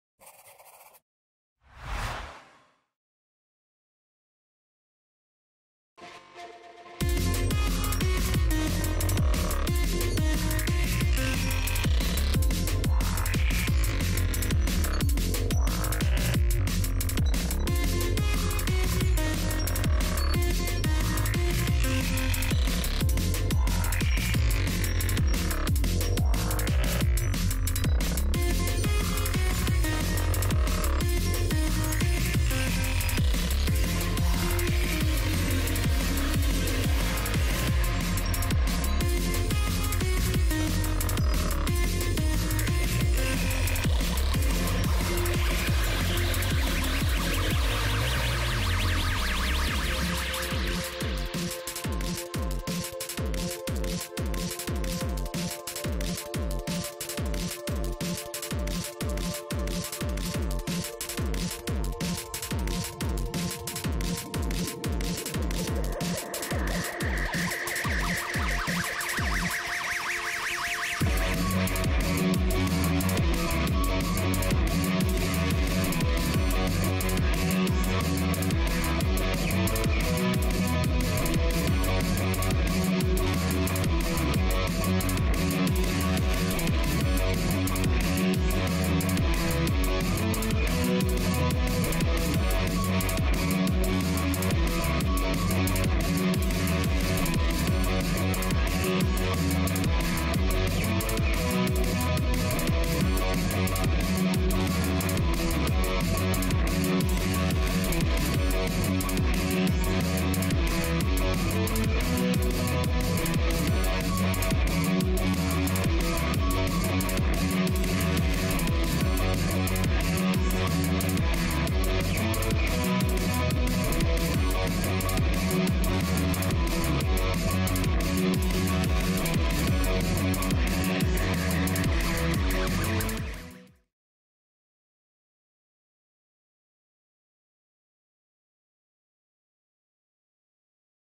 Time Lapse Abstract Pop Art Painting of Flea - Red Hot Chili Peppers
Time lapse art therapy speed painting of Flea from the Red Hot Chili Peppers